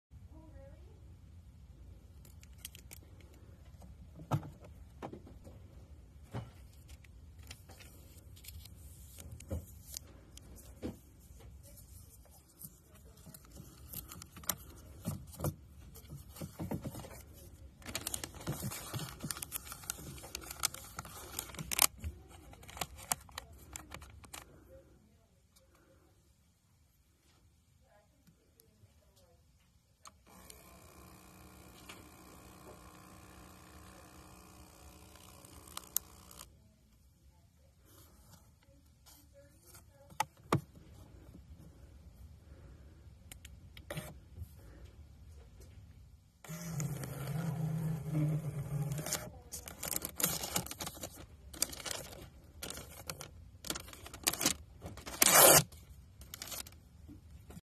How To Change The Paper in the Interacoustics Impedance AT235
A quick demonstration of how to change the printer paper in the tymp machine.